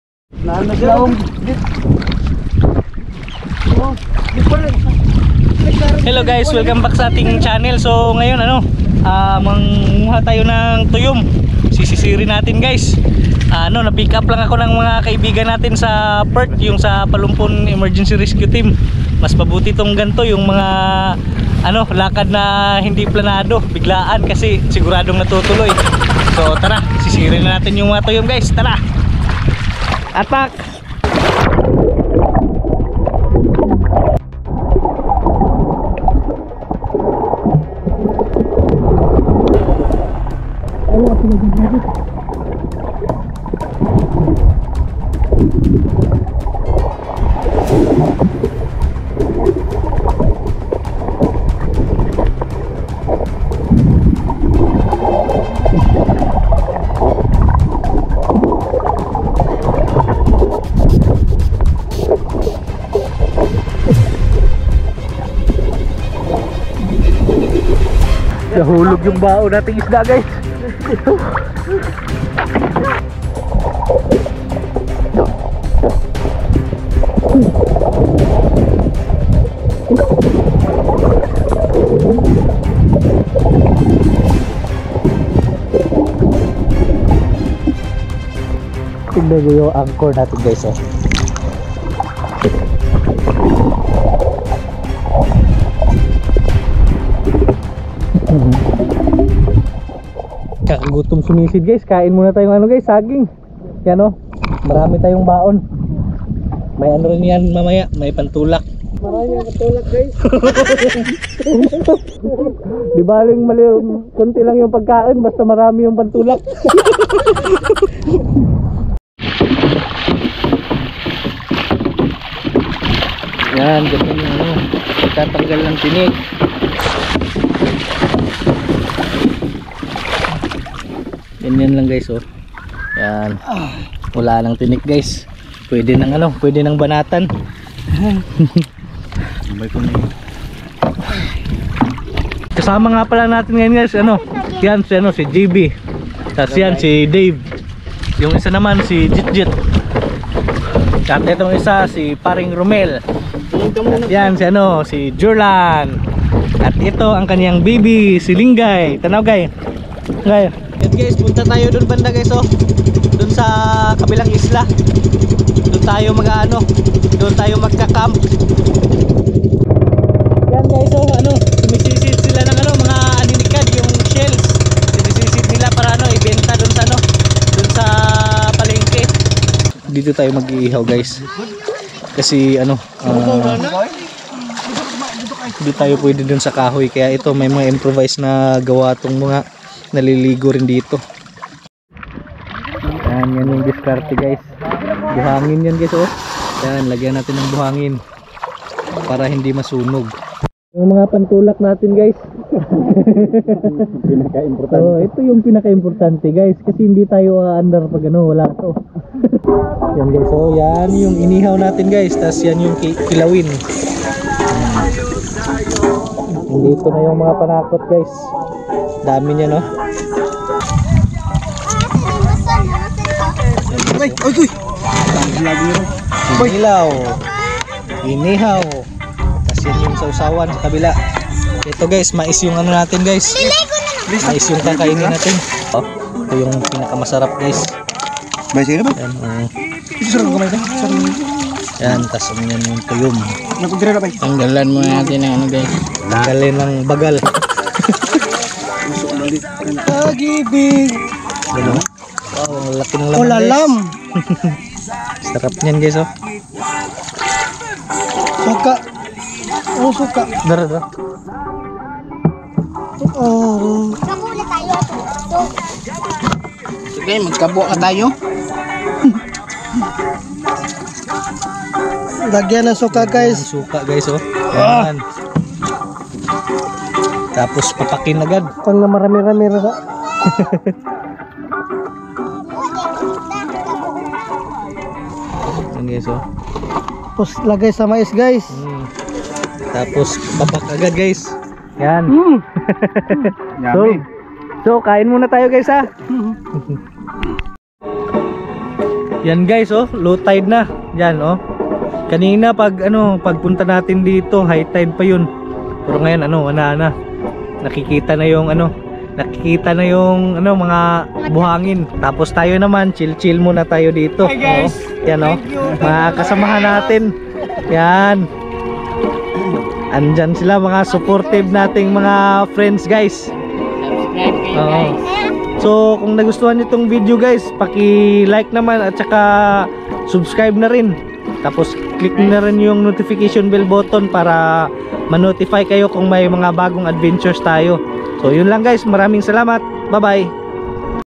Sea Urchin Feast: Unforgettable Uni Mukbang with Friends!
Join us for an epic Sea Urchin (Tuyom) Mukbang as we share delicious bites and laughter with friends! Dive into the unique flavors of freshly caught Uni and learn about its culinary appeal. Gather 'round for a fun, mouthwatering experience you won't want to miss!